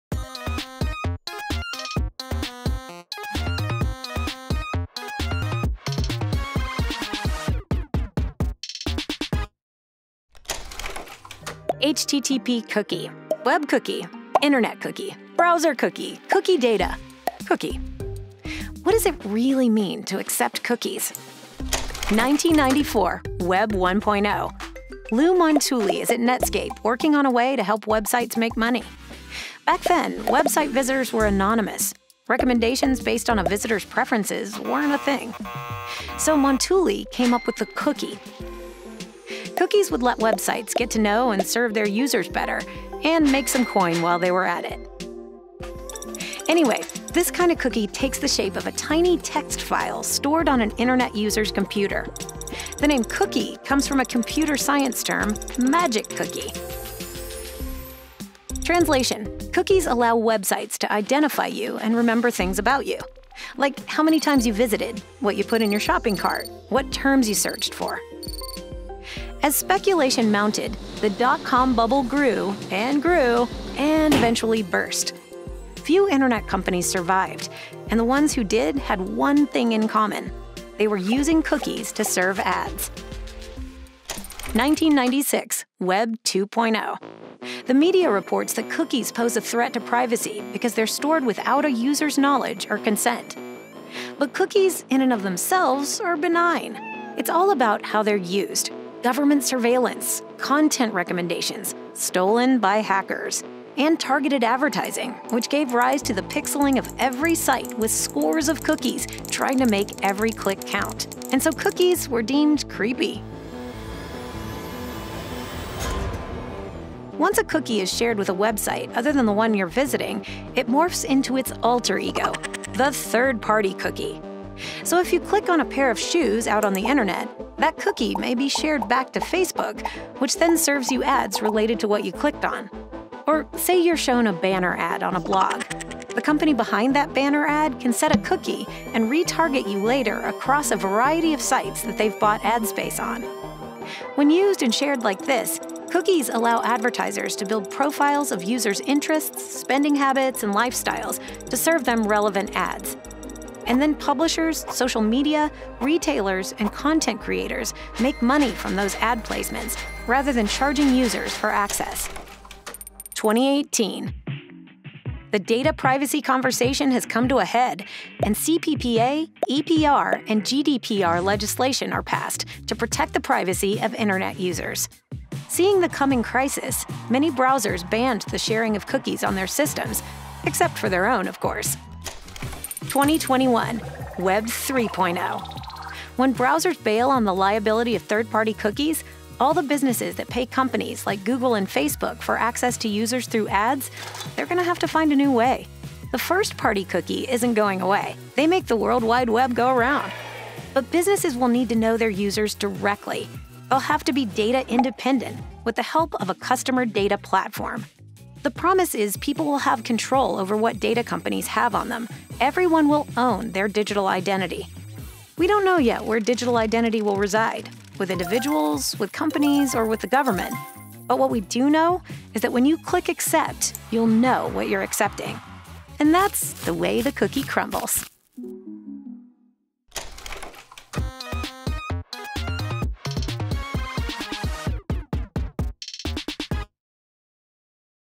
What Does it Really Mean to Accept Cookies?
In 2021, third-party cookies will be removed from all internet browsers due to the threat they pose to user privacy. For those companies who rely on them to reach their customers via advertising, they’ll now have no choice but to get to know their users directly.

Learn more about customer data privacy and how to reach your customers in a cookieless future